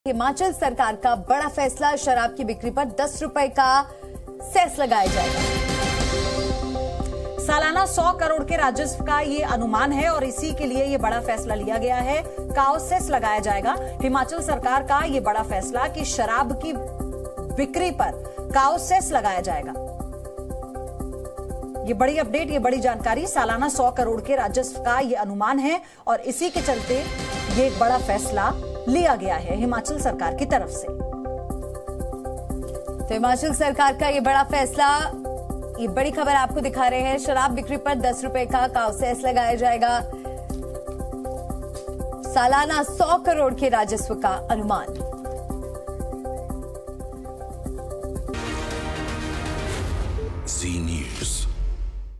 BREAKING NEWS: शराब की बिक्री पर हिमाचल सरकार का बड़ा फैसला, 10 रुपए का COW सेस लगाया जाएगा
शराब की बिक्री पर हिमाचल सरकार ने बड़ा फैसला लिया है। हिमाचल में अब शराब पर 10 रुपए का COW सेस लगाया जाएगा।

Himachal government has taken a big decision on the sale of liquor. Cow cess of Rs 10 will now be imposed on liquor in Himachal.